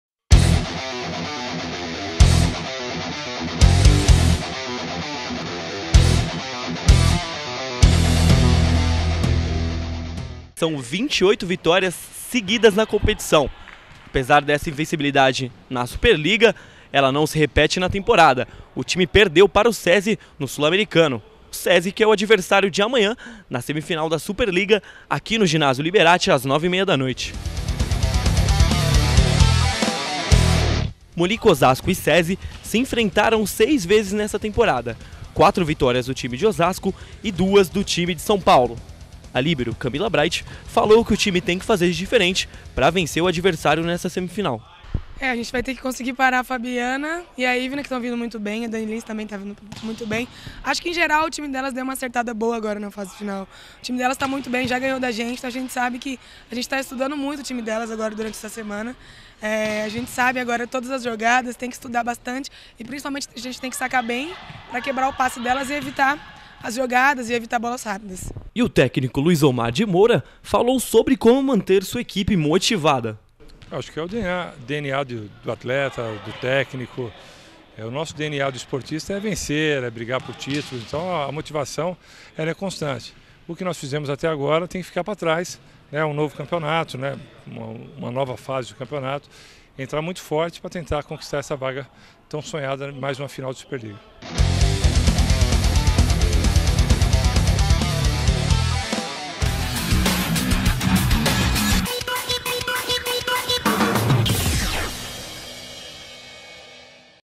Molico Osasco se prepara para decisão contra Sesi na Superliga
O Molico Osasco se prepara para a decisão nas semifinais contra o Sesi na Superliga feminina de vôlei, o time receberá no Ginásio José Liberatti a equipe e tenta a vaga na decisão da competição.

TV ESPORTE OSASCO